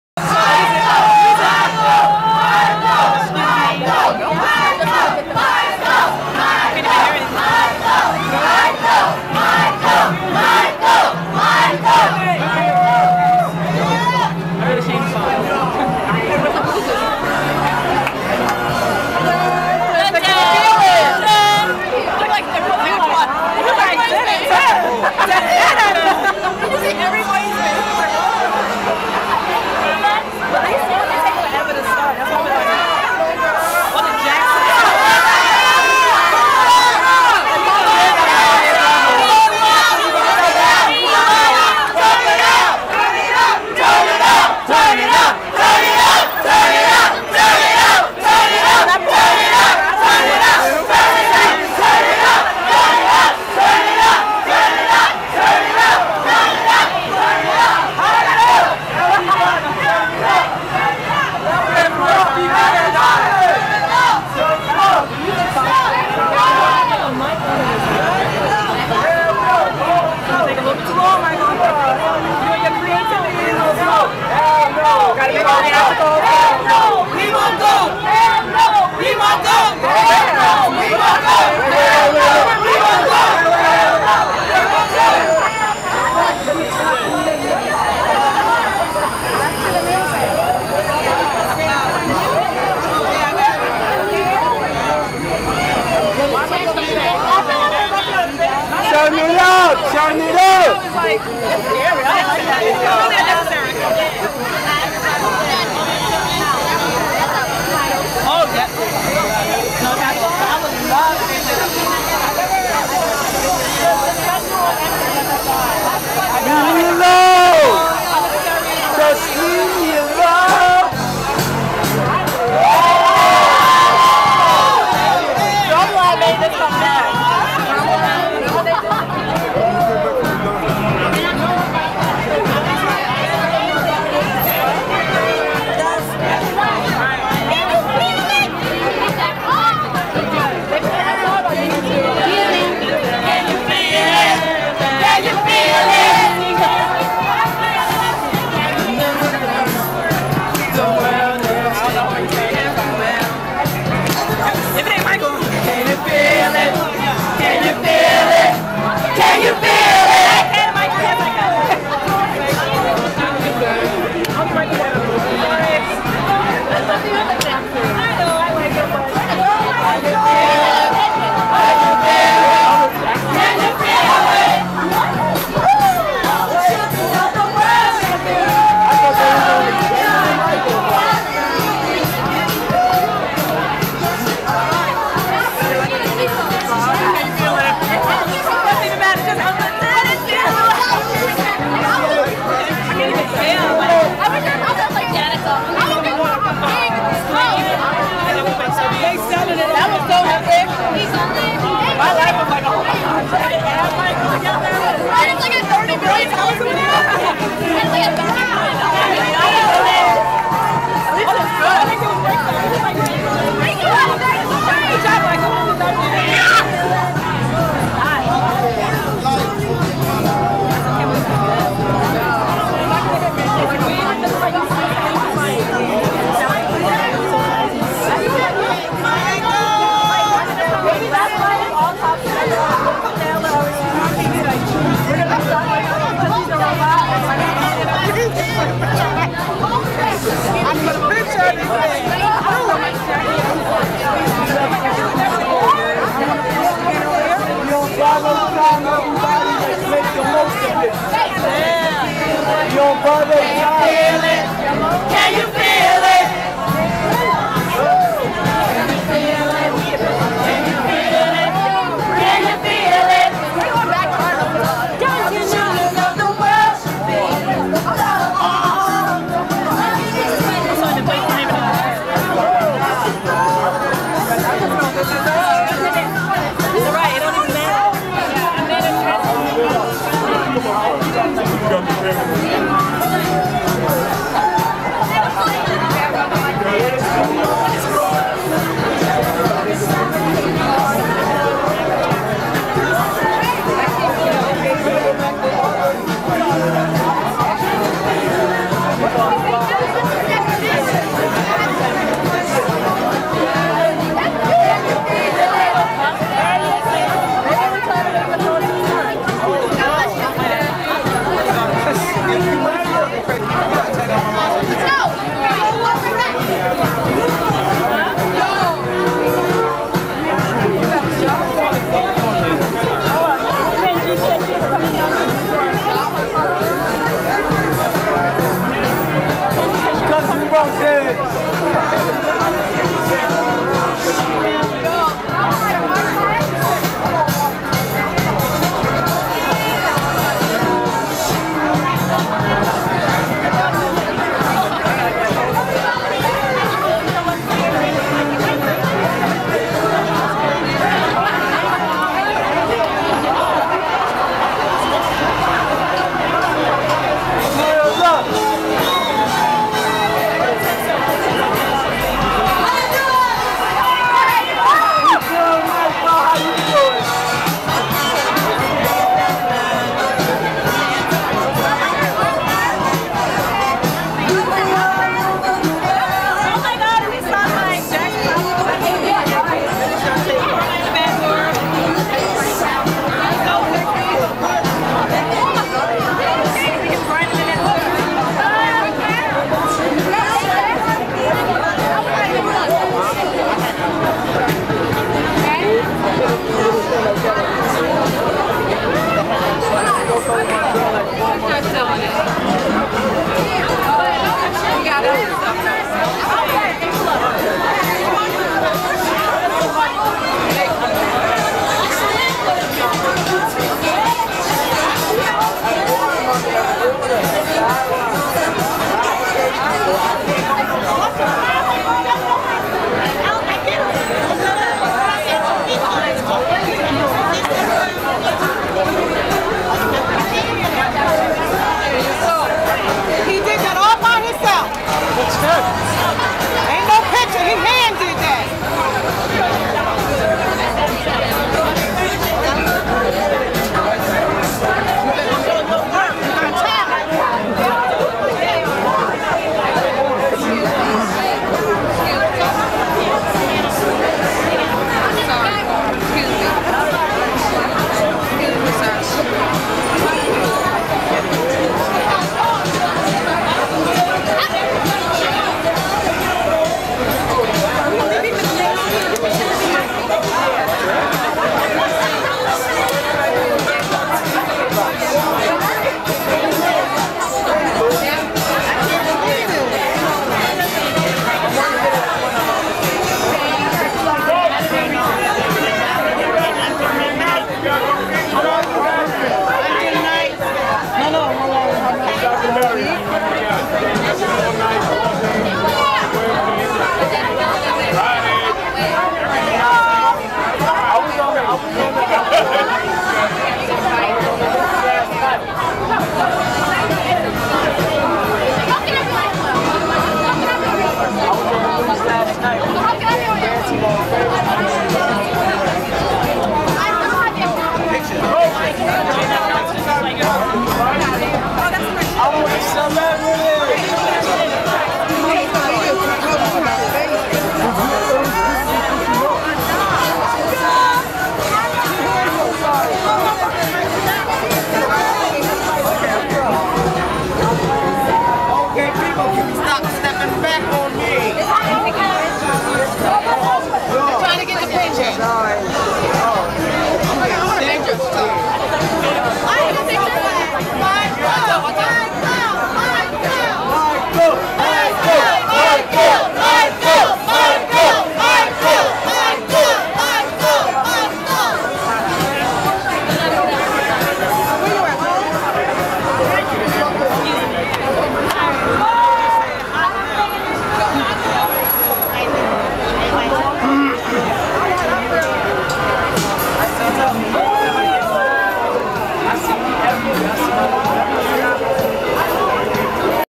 Michael Jackson Fans (4 of 8) at Apollo Theater in Harlem, New York City
Fans remembering Michael Jackson, Saturday afternoon, June 27, 2009